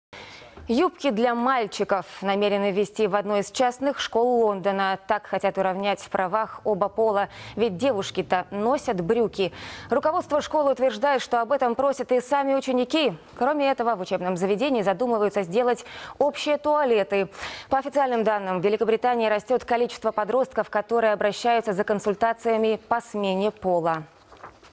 Юбки для мальчиков введут в одной из частных школ Лондона
Так хотят уравнять в правах оба пола, ведь девушки-то носят брюки. Руководство школы утверждает, что об этом просят и сами ученики. Кроме этого, в учебном заведении задумываются сделать общие туалеты. По официальным данным, в Великобритании растет количество подростков, которые обращаются за консультациями по смене пола.